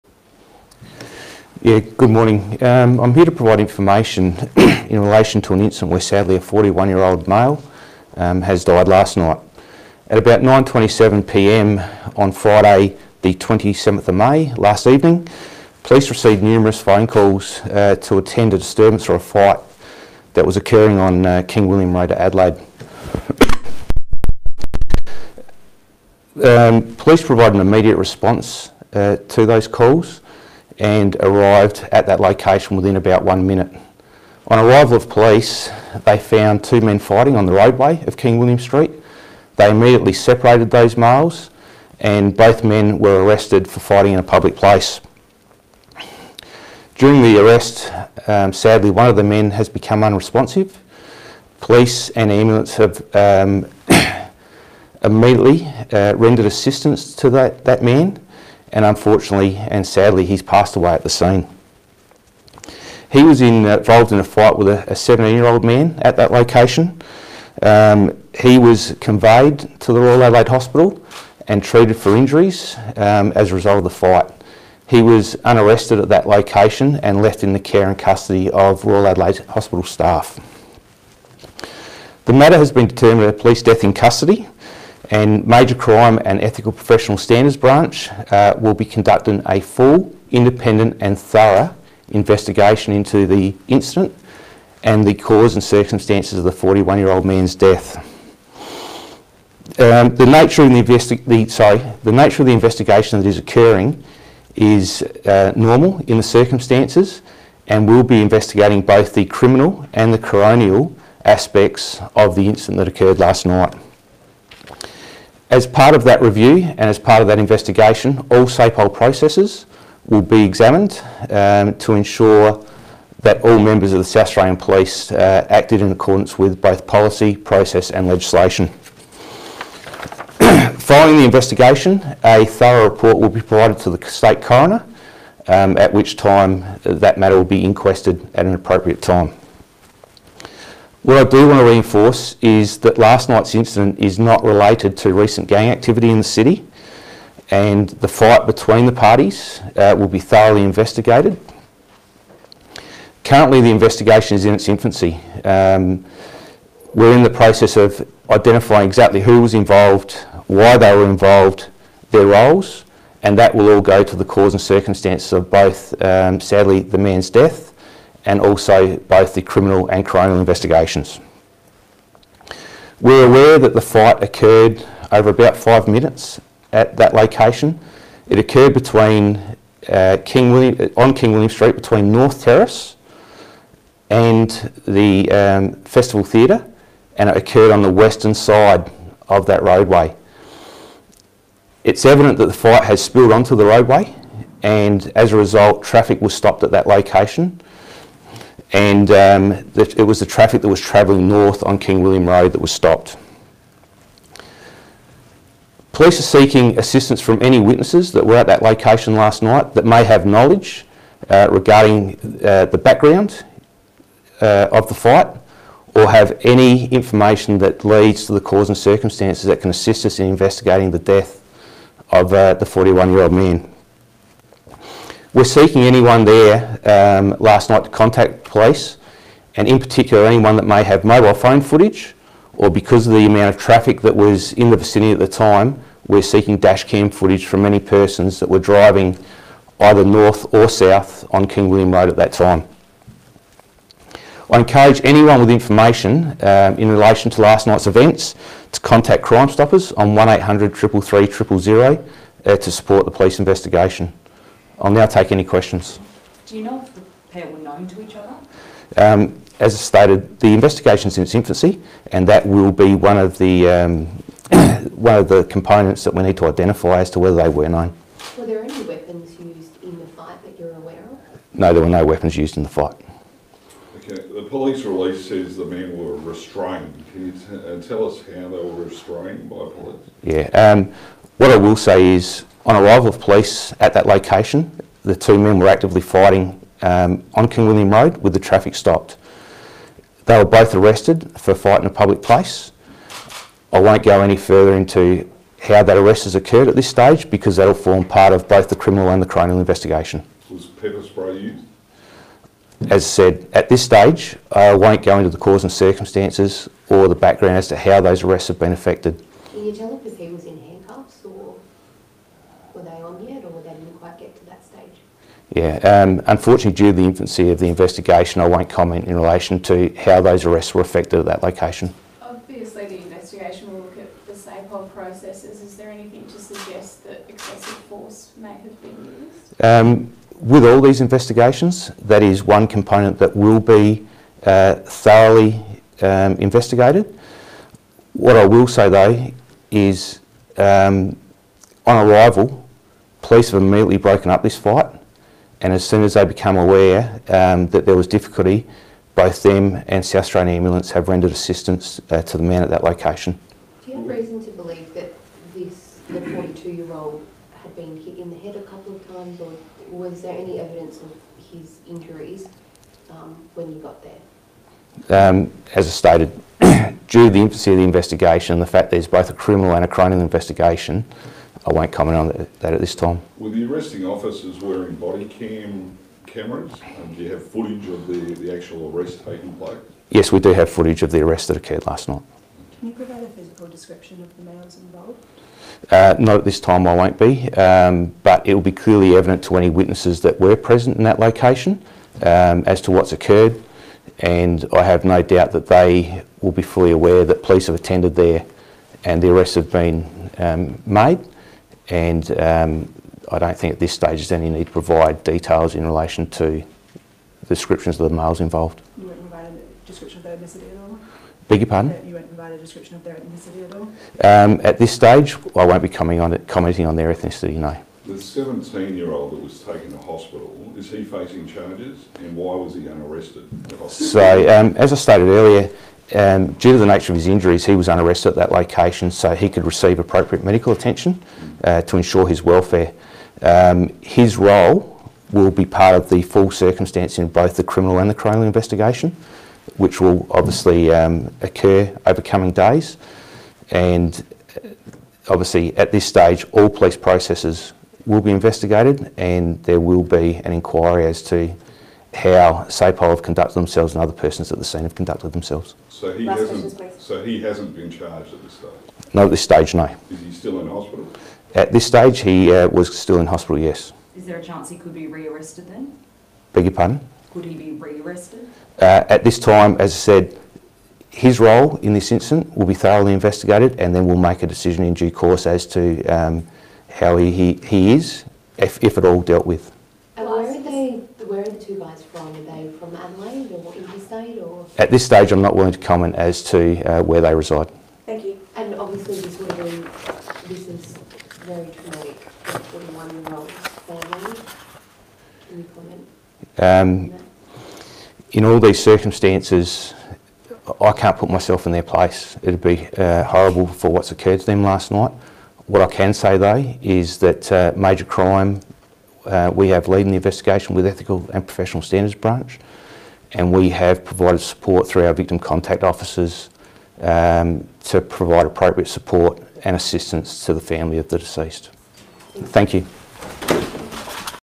Investigation into man's death on King William Road
Detective Inspector Brett Featherby regarding an investigation on King William Road.

Police are investigating the sudden death of a man on King William Road, Adelaide last night.  The man’s death has been determined to be a death in police custody.

At 9.27 pm on Friday 27 May, Police received numerous calls from members of the public reporting two males fighting on King William Road, Adelaide, adjacent to Parliament House.

Police attended immediately and found both males actively fighting.  The fight had spilled onto the roadway and was blocking northbound traffic.  Both males were restrained and placed under arrest.

During the arrest, one of the men, a 41-year-old, became unresponsive.

Police and paramedics commenced CPR, but sadly the man was pronounced deceased at the scene.

The other male, a 17-year-old, was taken by ambulance to the Royal Adelaide Hospital for treatment of injuries sustained in the fight.

Major Crime and Ethical and Professional Standards detectives are investigating the circumstances surrounding the incident, and a report will be prepared for the Coroner.

The investigation is in its early stages and is ongoing.

Any members of the public who witnessed this incident or may have phone or dashcam footage are encouraged to contact Crime Stoppers on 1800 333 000